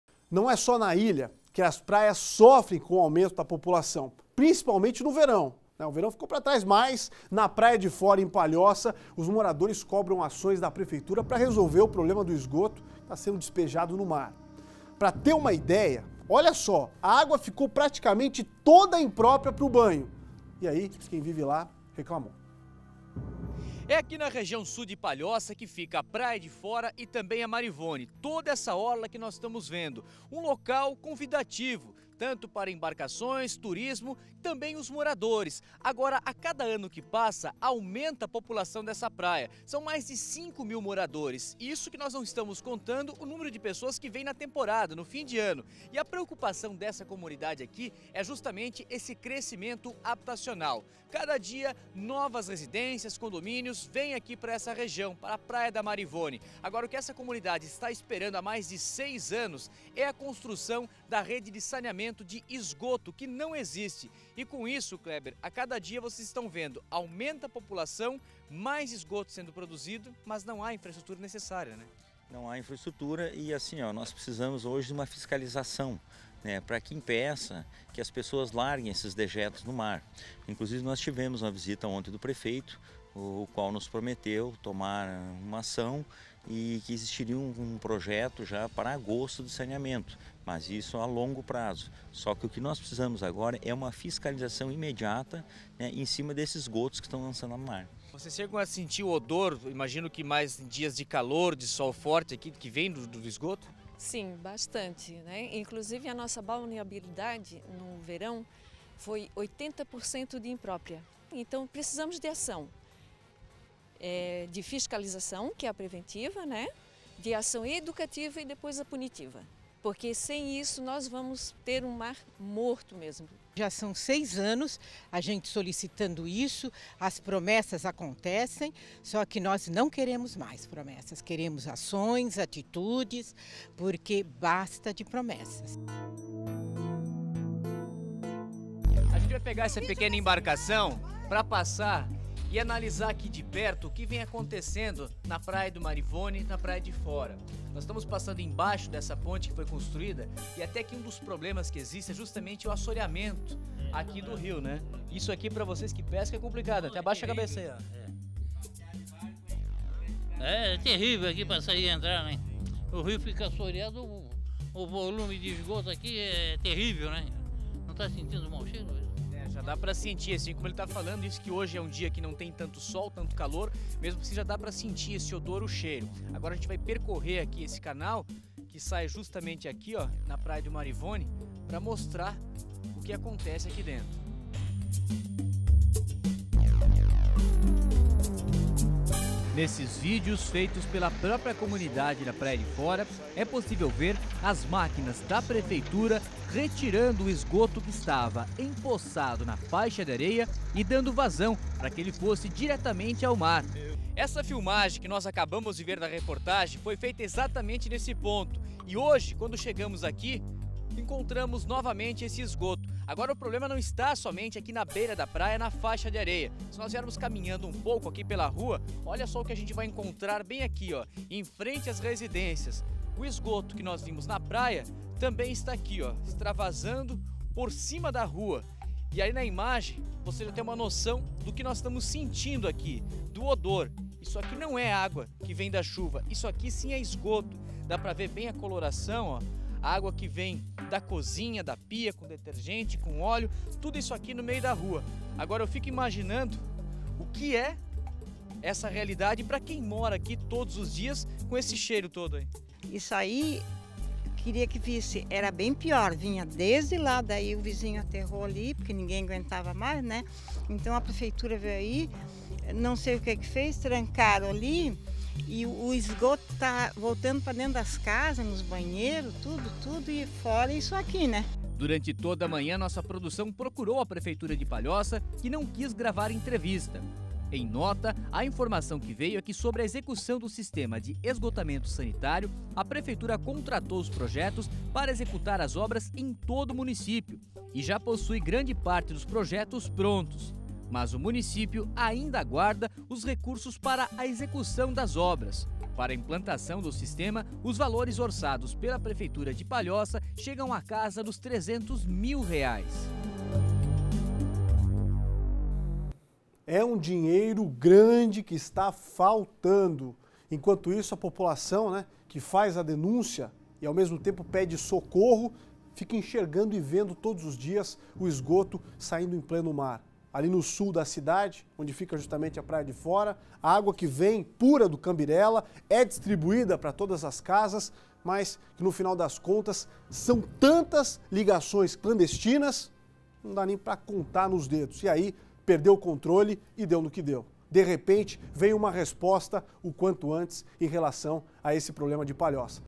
Moradores da Praia de Fora cobram fiscalização de esgoto no mar
Na Praia de Fora, em Palhoça, os moradores cobram ações da prefeitura pra resolver o problema do esgoto que é despejado no mar.  A água ficou praticamente toda imprópria para banho.